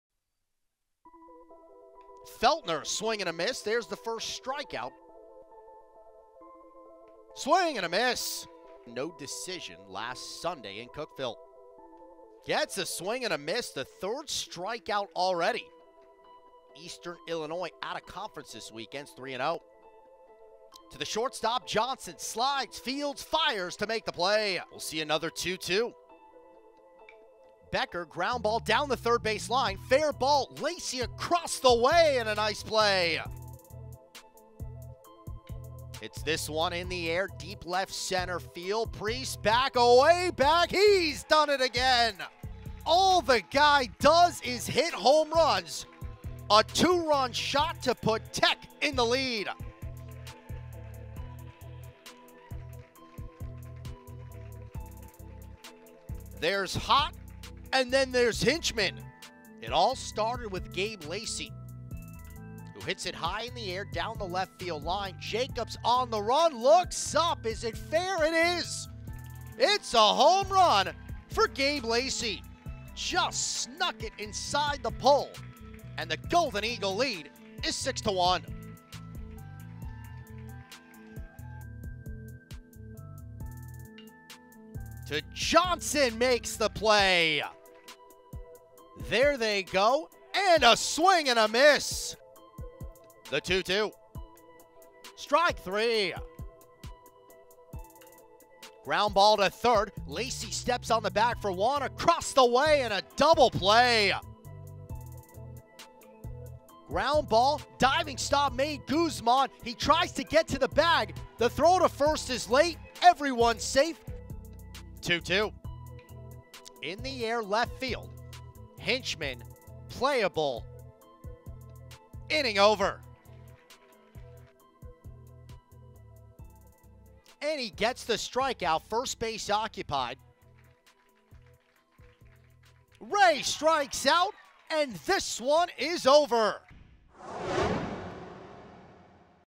Highlights: 04/03/22 Tennessee Tech Baseball vs Morehead State
Final:
TTU: 8
MOR: 6